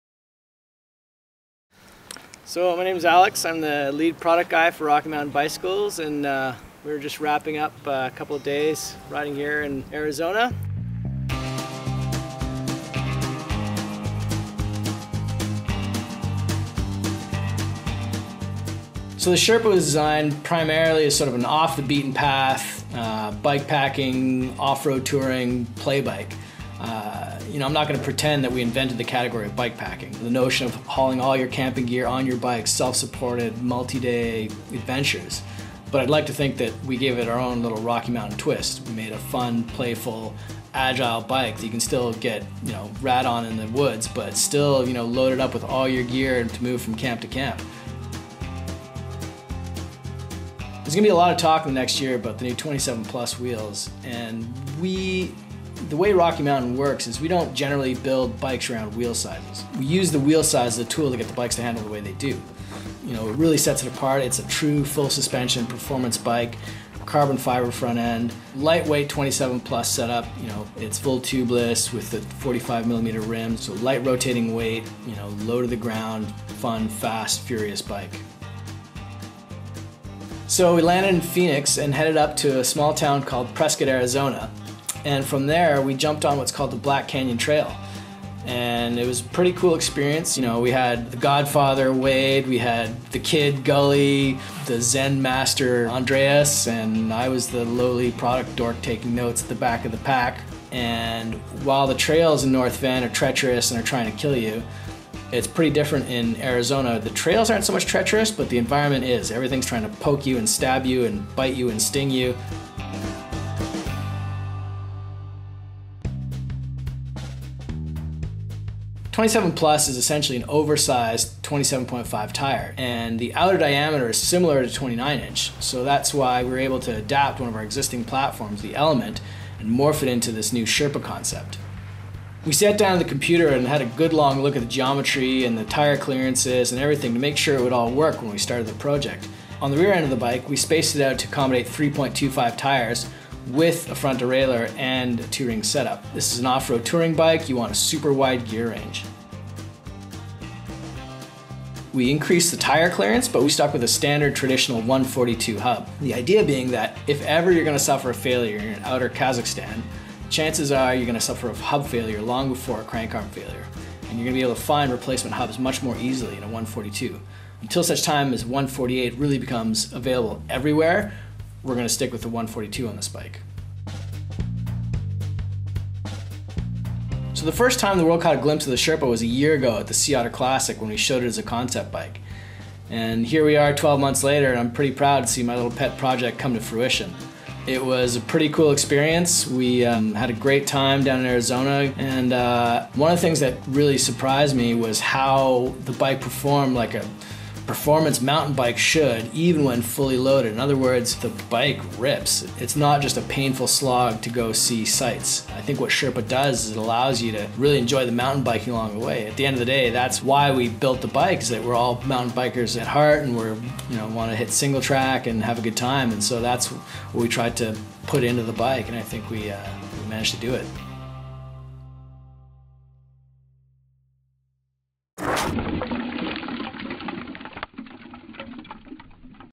Introducing the Rocky Mountain Sherpa
We're excited to announce the new Sherpa overland bike. Bikepacking has been around for a long time, but the Sherpa puts a Rocky Mountain twist on the concept. We wanted a bike that could tackle more challenging terrain and would let us get a little rowdy—even loaded down for multi day self-supported adventures. The Sherpa will carry you and your gear to the ends of the earth, far from the nearest Strava segment and through whatever ambitious backcountry route you didn’t know you were planning.